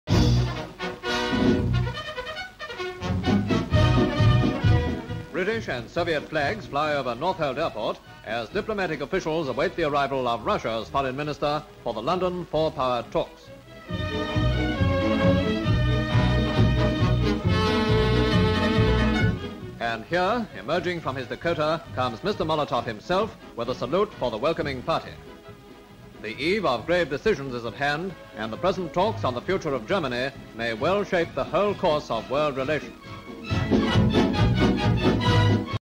POLITICS: Molotov arrives in London for Four Power talks (1947)
Soviet Commissar for Foreign Affairs Vyacheslav Molotov arrives for Four Power talks soon to begin in London.

Full Description:

SLATE INFORMATION: The Foreign Ministers meet in London

ENGLAND: London: Northolt Airport:
British and Soviet flags flying
Plane carrying Vyacheslav Molotov (Russian Foreign Minister) lands
Molotov emerges from Dakota plane and salutes welcoming party
Molotov into car and departs
Background: Soviet Commissar for Foreign Affairs Vyacheslav Molotov arrives for Four Power talks soon to begin in London.